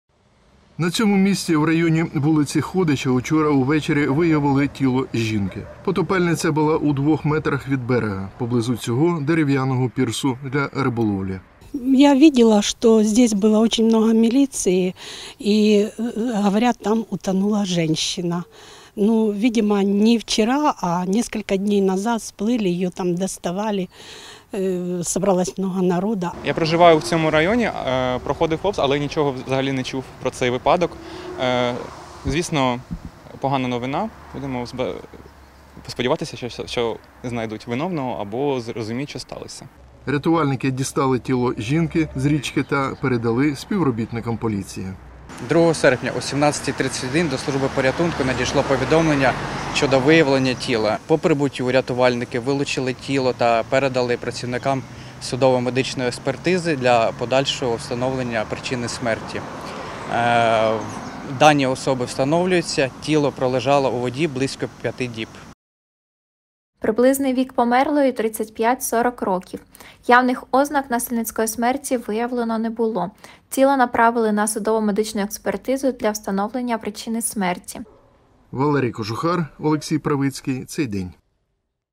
З річки дістали тіло потопельниці: поліція встановлює особу загиблої
Поліція встановлює особу жінки, тіло якої виявили у Центрально-Міському районі, у річці Саксагань. Потопельниці років 30-40, причини смерті жінки з'ясовують правоохоронці.